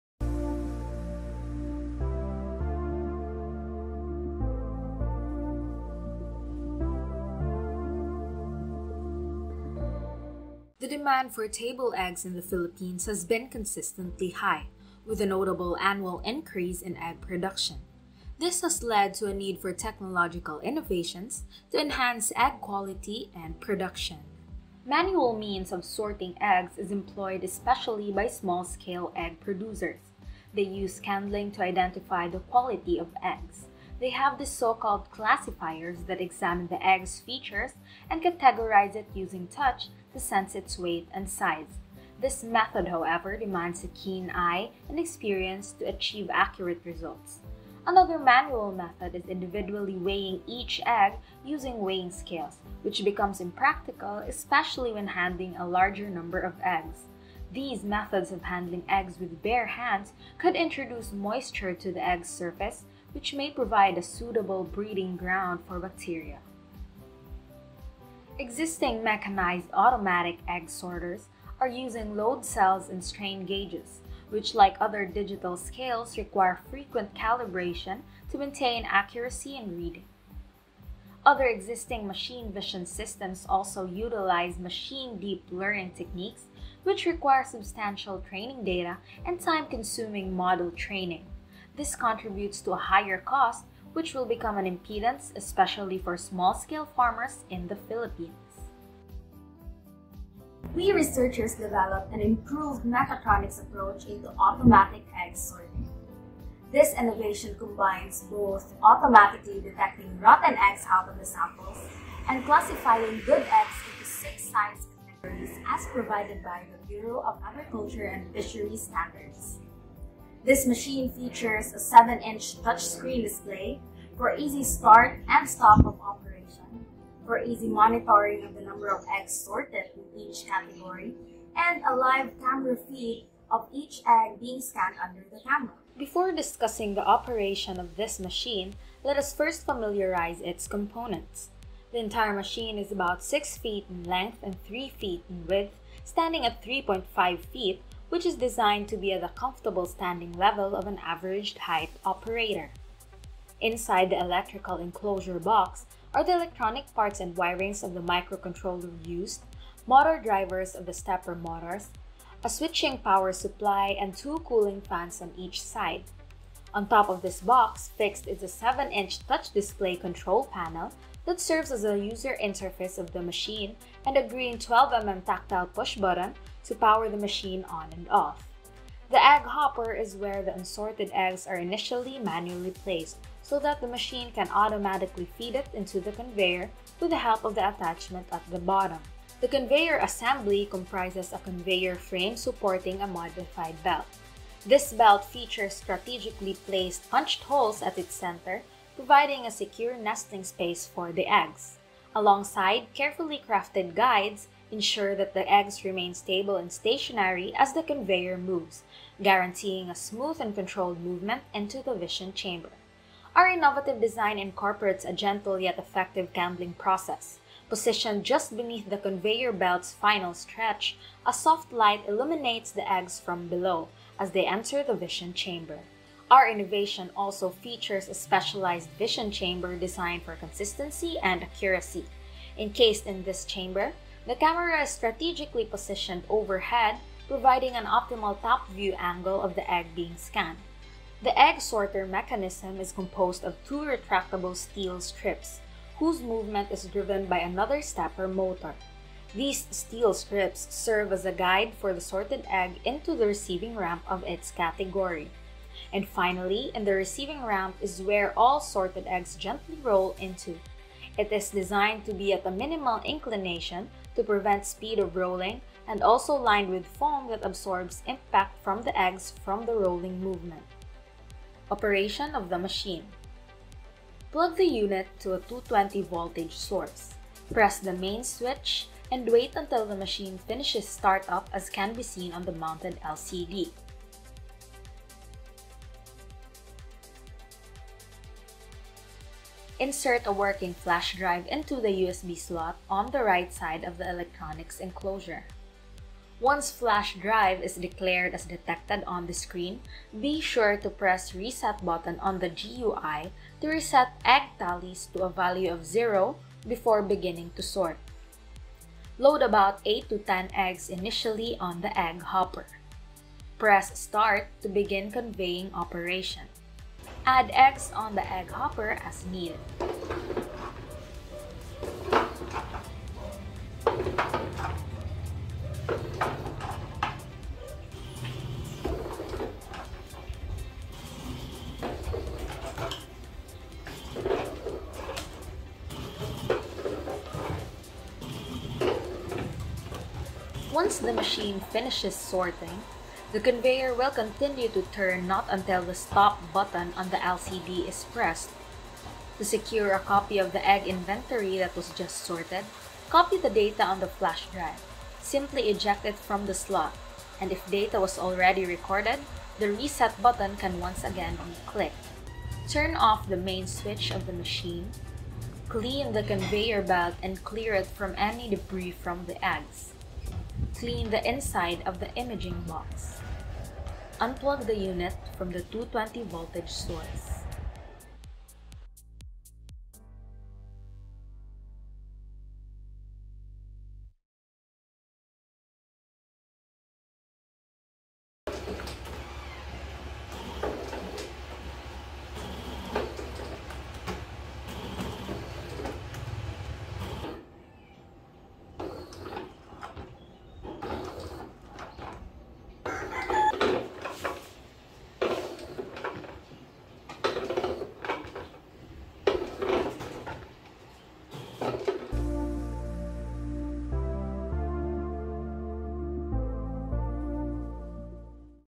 2023 PAES Undergraduate Engineering Design Contest Entry- Cebu Institute of Technology University
Entry for the 2023 PAES Undergraduate Engineering Design Contest:

DEVELOPMENT OF MECHATRONICS APPROACH FOR PHILIPPINE TABLE EGG-GRADING INTEGRATED WITH ROTTEN EGG SORTER UTILIZING BINARY IMAGE PROCESSING

Proponents:
Batiancila, Kent John
Inocellas, EJ Kenth
Maligro, Glenysse Gale
Malingin, Gabriel
Yray, Maris